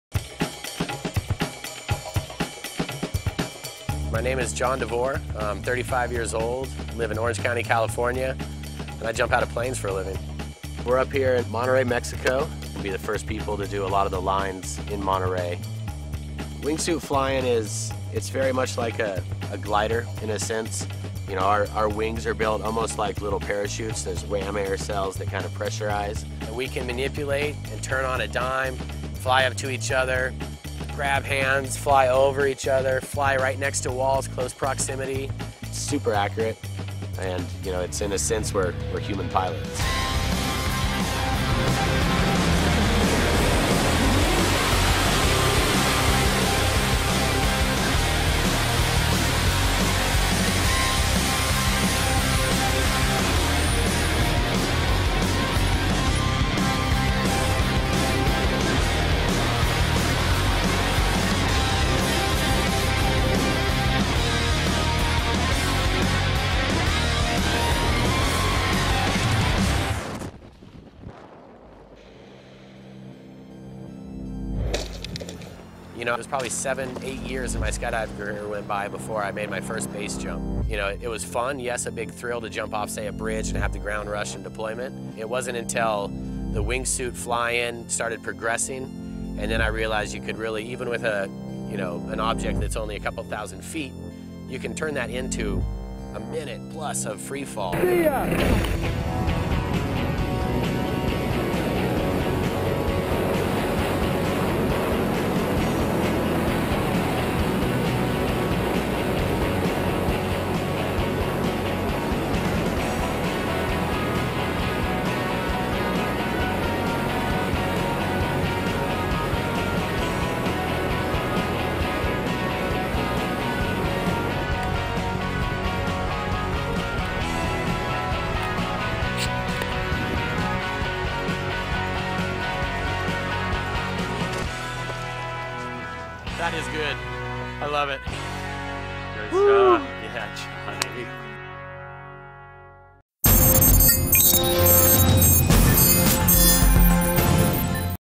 Those Amazing Men with Wiings Explore Mexico
Jon Devore and Mike Swanson on a trip to Mexico to explore and fly their wingsuits on the two top iconic mountains in Monterrey: Cerro de la Silla and La Huasteca.